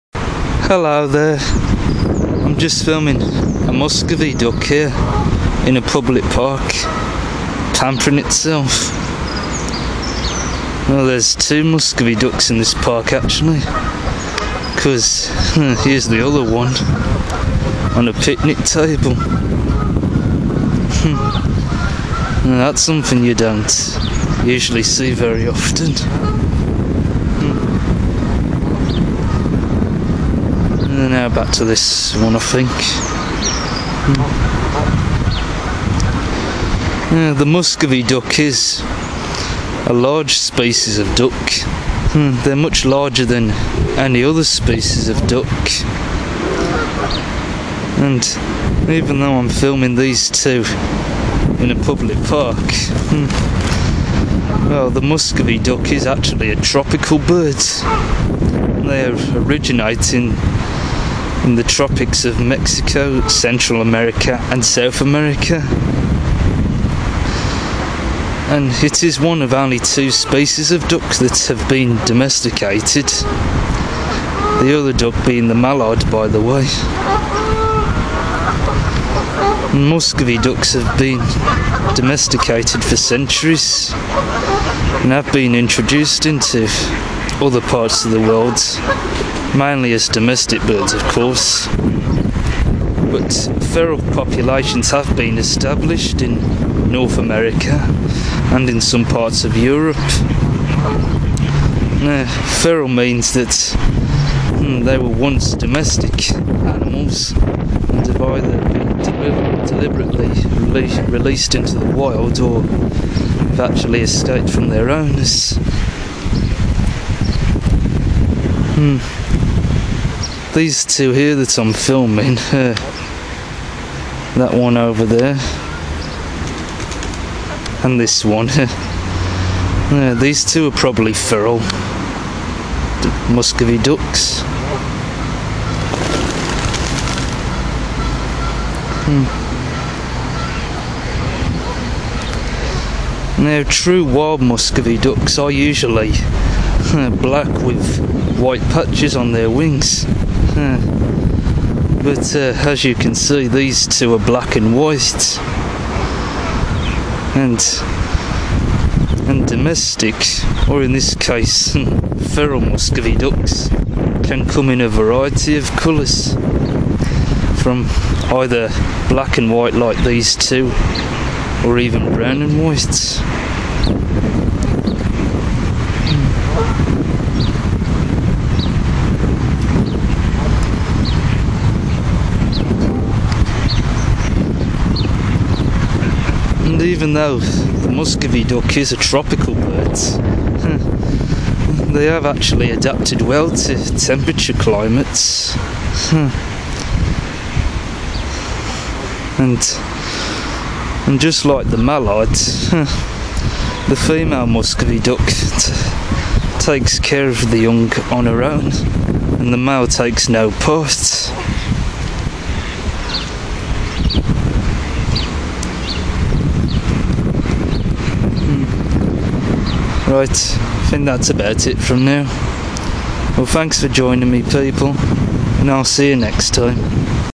A.K.5000 Muscovy Duck
Here's another waterfowl video guys, this time it's the Muscovy duck. I filmed these two Muscovy ducks in a public park not far from where I live, but these ducks are actually native to Mexico, Central and South America. Like the mallard (Anas platyrhynchos) it has also been domesticated and introduced all around the world; and do adapt well to colder climates. Some domesticated ducks have escaped into the wild and now breed outside their native range, including in Western Europe, Canada and the USA. True wild Muscovy ducks are quite different to domesticated ducks. They are blackish, with iridescent green and purple dorsal plumage and with white wing patches. On the head, the male has a short crest on the nape. The bill is black with a speckling of pale pink. A blackish or dark red knob can be seen at the bill base, and the bare skin of the face is similar to that in colour. The eyes are yellowish-brown. The legs and webbed feet are blackish with long claws. The wild female is similar in plumage, but is also much smaller, and she has a feathered face and lacks the prominent knob. The juvenile is duller overall, with little or no white on the upperwing. Domestic Muscovy ducks come in a variety of plumages including chocolate brown, blue or pied (like these two in the video).  The wild Muscovy duck normally inhabits forested swamps, lakes, streams and nearby grassland and farm crops, and often roosts in trees at night. Its diet consists of plant material obtained by grazing or dabbling in shallow water, and small fish, amphibians, reptiles, crustaceans, insects, and millipedes. As I said earlier, I filmed these two Muscovy ducks in the park, one was on the grass and the other was on a picnic table.

Other Facts

Kingdom: Animalia
Phylum: Chordata
Class: Aves
Order: Anseriformes
Family: Anatidae
Scientific Name: Cairina moschata
Length: 66-84 cm
Wingspan: 137-152 cm
Weight: 1.1-4.1 kg
Number of Eggs: 8-16
Incubation Period: 33-35 days
Fledging Period: 60-70 days
Lifespan: Up to 20 years
Distribution: Wild Muscovy ducks exist in Mexico, Central America & South America; whereas feral Muscovy ducks exist in parts of North America & Western Europe.
Conservation Status: Least Concern

All facts and information are based on what I've researched on the internet or read about in books or heard from other animal experts when I travel to other different countries around the world.